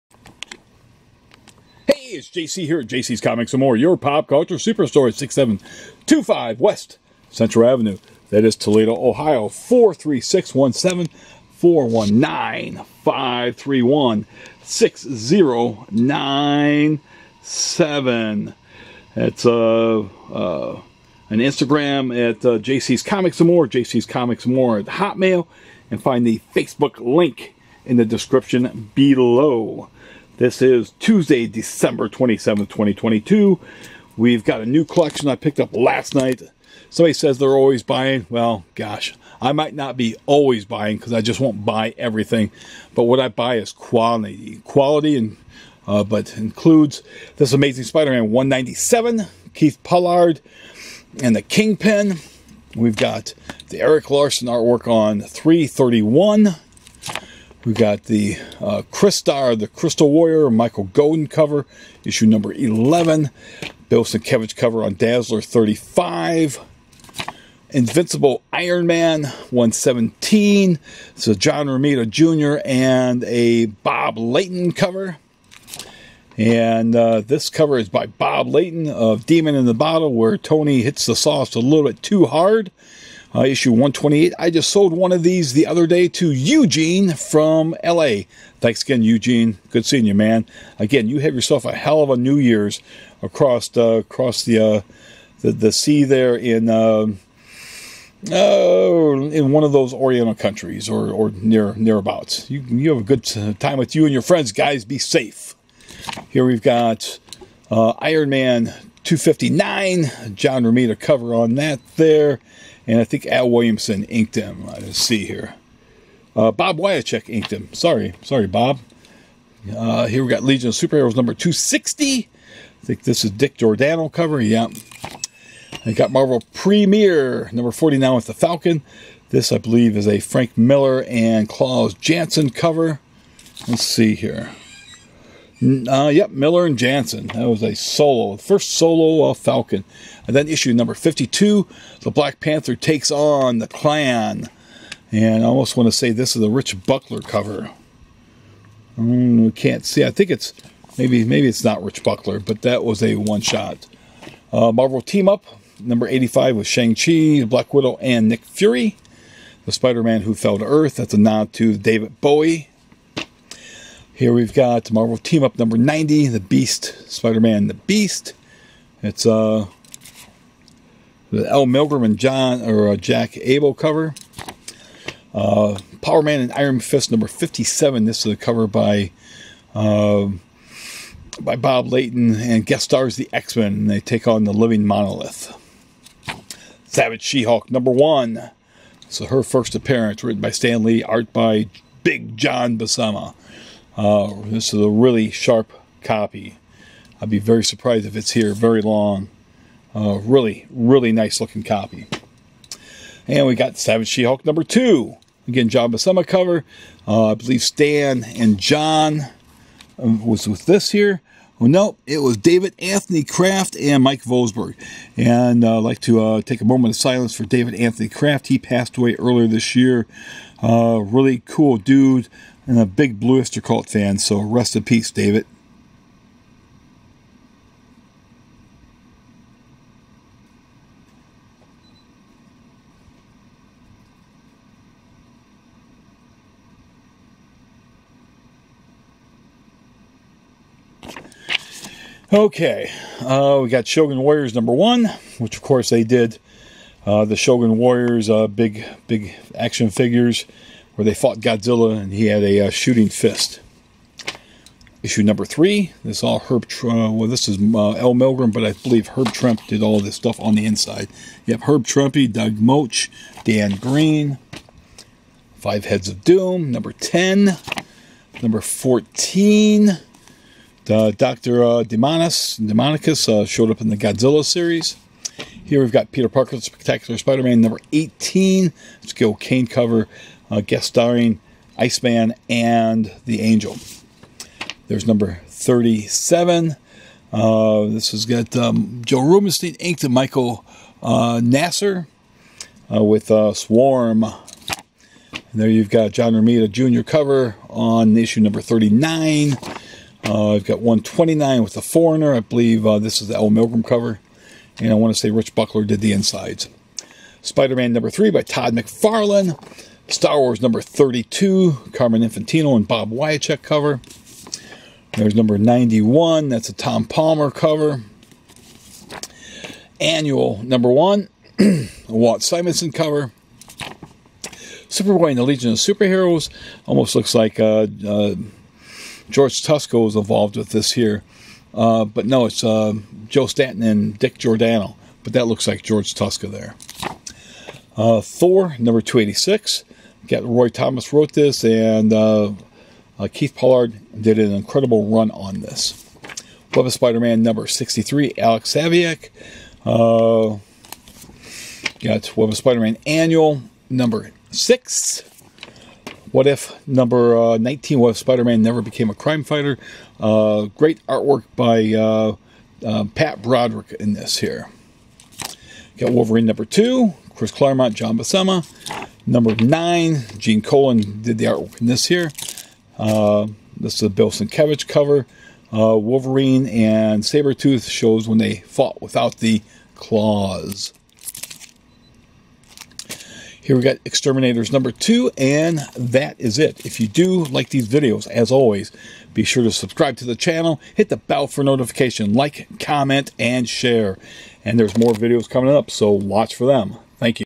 New Collection Savage She-Hulk 1 Iron Man 128 Wolverine 10 What if Thor MTU @ JC'S Comics N More
boblayton ironman shehulk This is video 1426 December 27, 2022 A new collection @ JC'S Comics N' More: Your Pop Culture Super-Store 6725 W Central Ave Toledo, OH 43617 , e-mail & Follow JC'S on Instagram @ jcscomicsnmore.